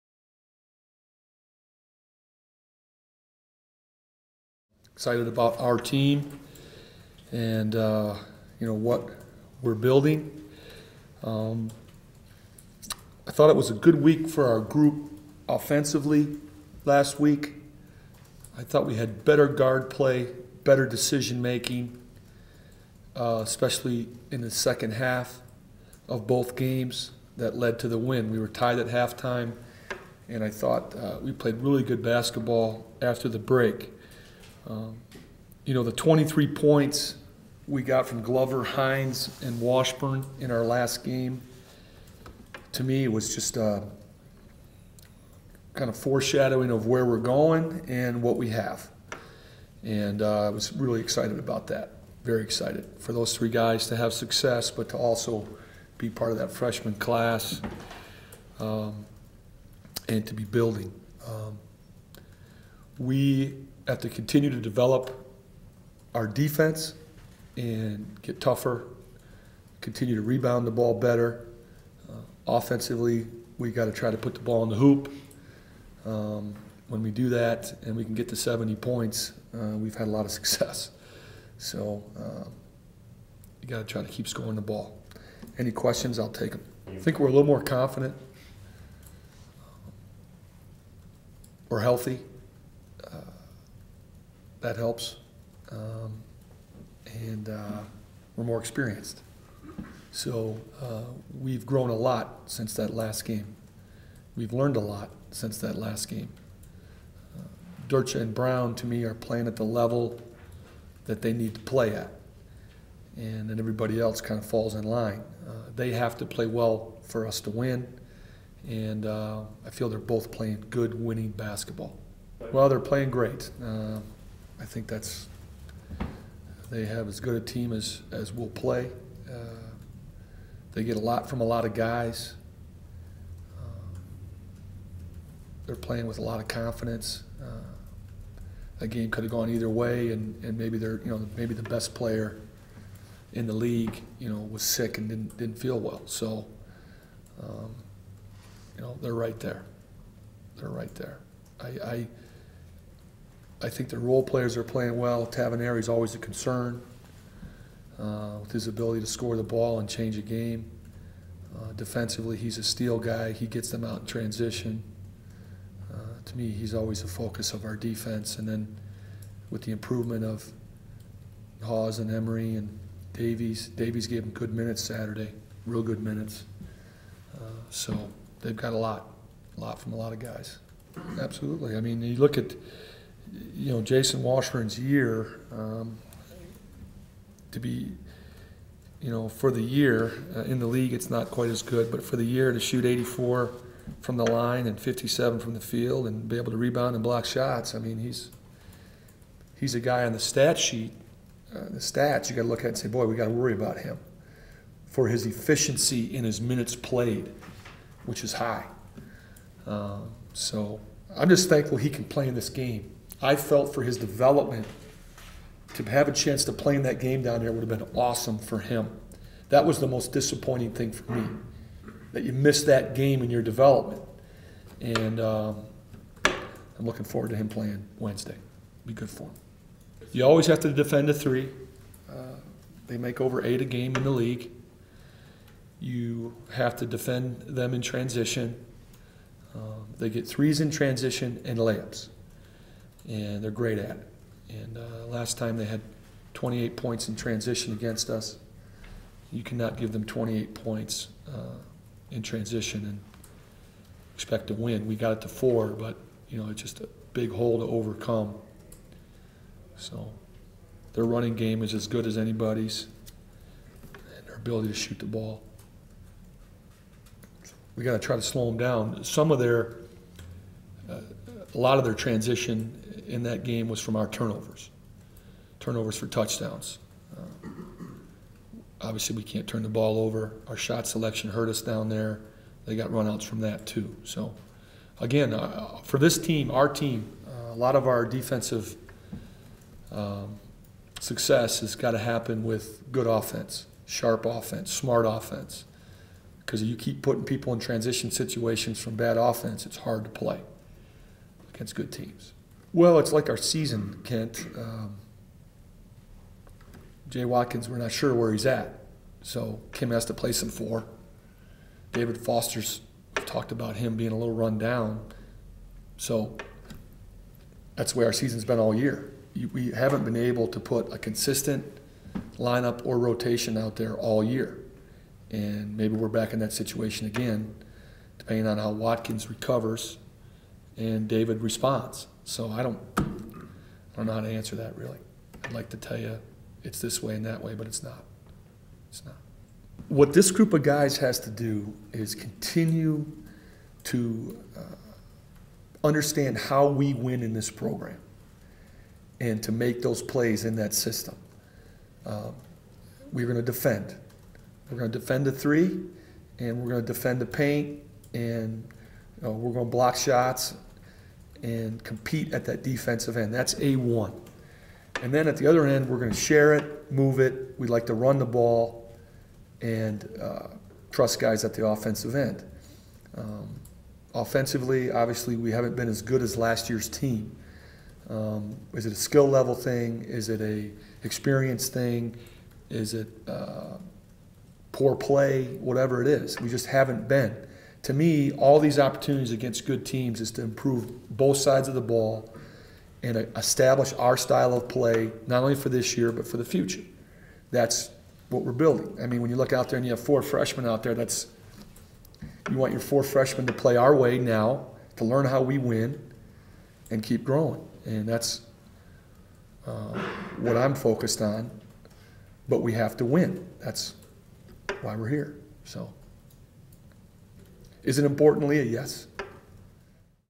University of Utah - Men's Basketball Press Conference - 3/01/10
University of Utah - Men's Basketball Press Conference with Coach Jim Boylen - 3/01/10